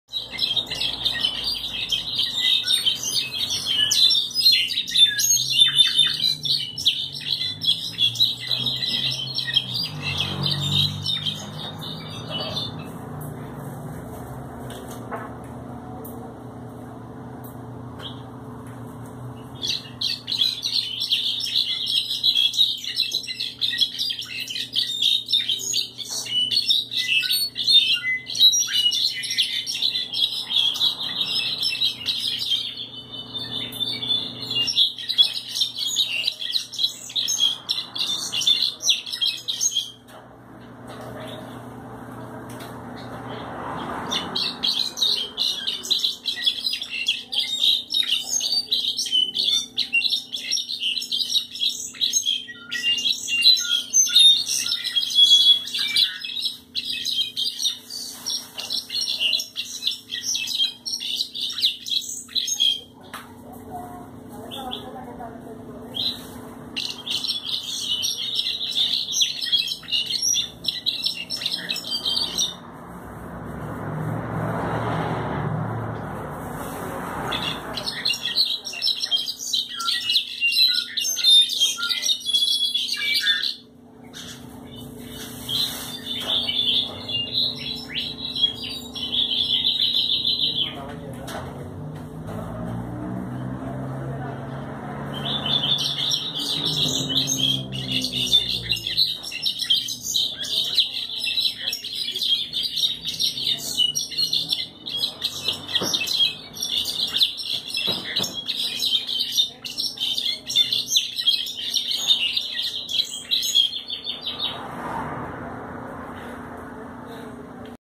Jilguero Cantando (pinto 2)
Este es el canto del que era el Jilguero Pinto número 2.